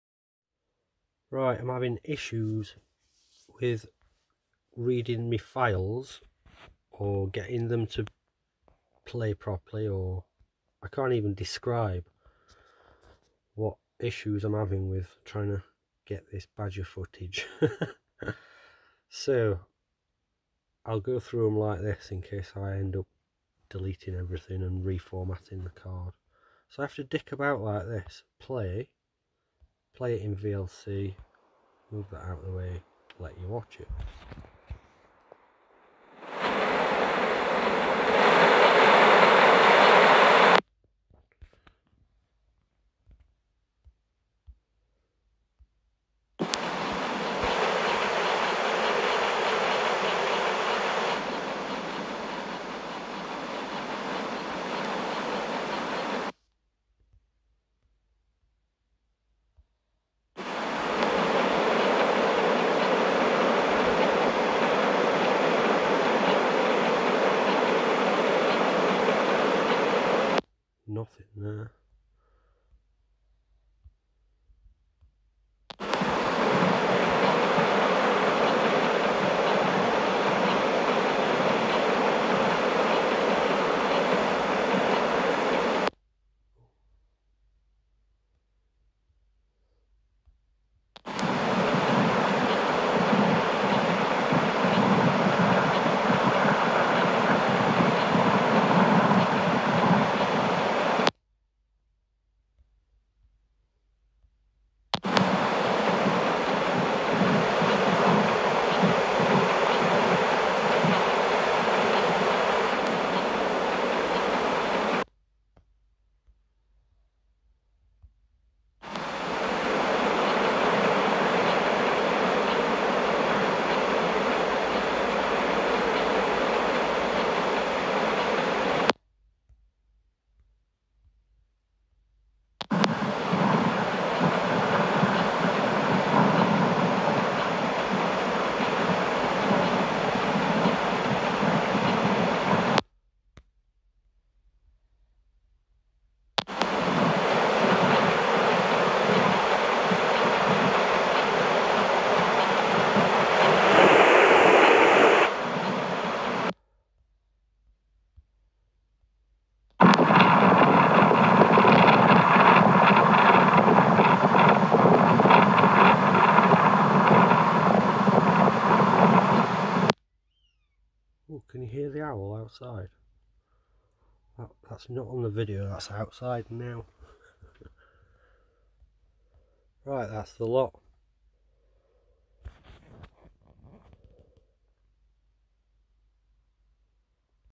Badger footage... in an awkward way....
having some issues with reading videos,  transferring videos from the cam card to the phone. so did this crap video and will re format the card to see if that helps.